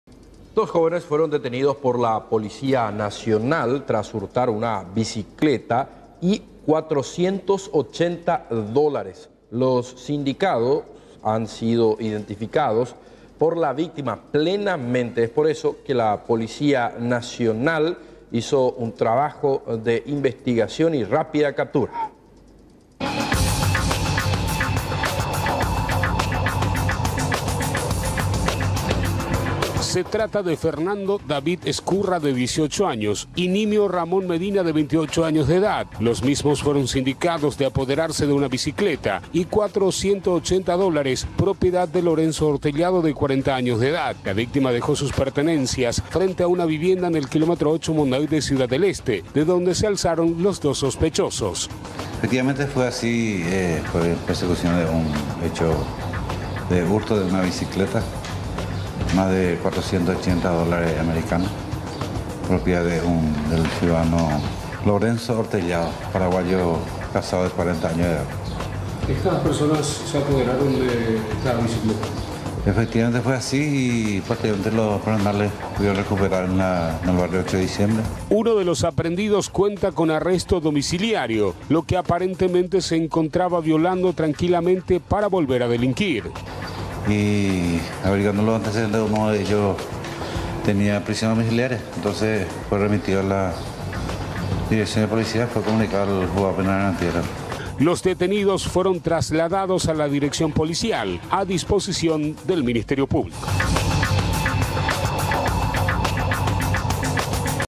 Capturan a dos jóvenes por robar una bicicleta
Los detenidos fueron plenamente identificados por la víctima

Las autoridades policiales informaron que los supuestos delincuentes, también se robaron más de 400 dólares.